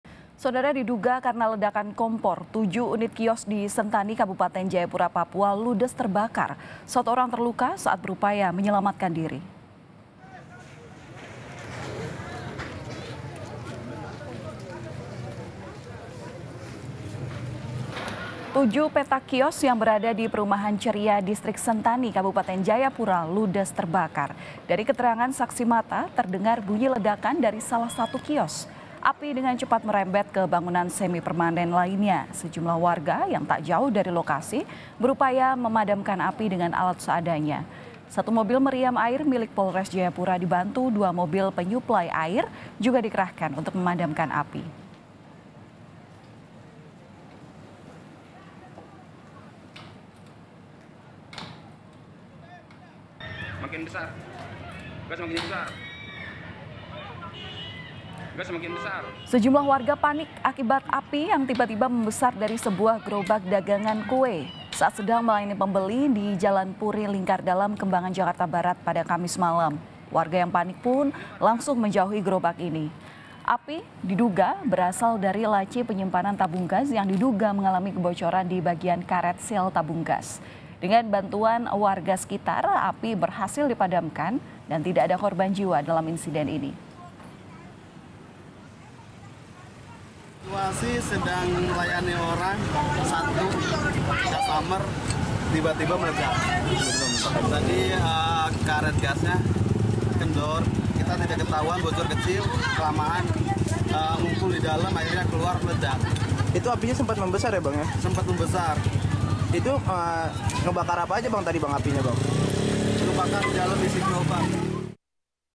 Diduga Akibat Kompor Meledak, Tujuh Unit Kios di Sentani Ludes Terbakar!
KOMPAS.TV - Tujuh petak kios yang berada di Perumahan Ceria, Distrik Sentani, Kabupaten Jayapura, Papua ludes terbakar.

Dari keterangan saksi mata, terdengar bunyi ledakan dari salah satu kios. Api dengan cepat merembet ke bangunan semipermanen lainnya.

Baca Juga Mobil Es Krim Terbakar di Subang, Sopir Berhasil Selamatkan Diri di

Sejumlah warga yang tak jauh dari lokasi berupaya memedamkan api dengan alat seadanya.

Satu mobil meriam air milik Polres Jayapura dibantu dua mobil penyuplai air juga dikerahkan untuk memadamkan api.

Tidak hanya di Jayapura, Papua, di Jakarta sejumlah warga panik akibat api yang tiba-tiba membesar dari sebuah gerobak dagangan kue saat sedang melayani pembeli di Jalan Puri Lingkar Dalam, Kembangan, Jakarta Barat, Kamis (12/01/23) malam.

Warga yang panik langsung menjauhi gerobak.

Api diduga berasa dari laci penyimpanan tabung gas yang diduga mengalami kebocoran di bagian karet seal tabung gas.

Dengan bantuan warga sekitar, api berhasil dipadamkan dan tidak ada korban jiwa dalam insiden ini.